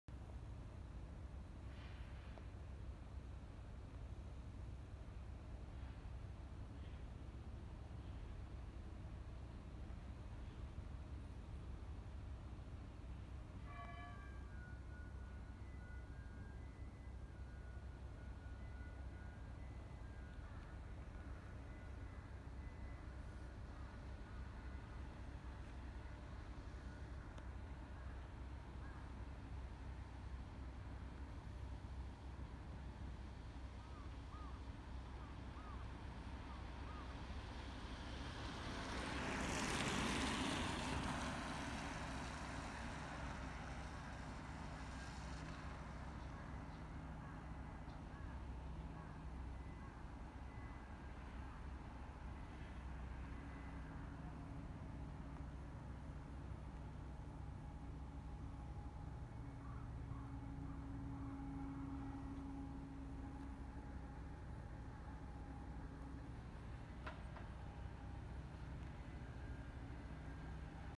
北広島に建設中のファイターズ新スタジアム (2021年11月)
新庄監督が就任することで話題の北海道日本ハムファイターズが、北広島市に建設中の新ボールパークの2021年11月時点の様子です。2023年に完成予定だそうです。新千歳空港と札幌駅とを結ぶ列車の中からも見えます。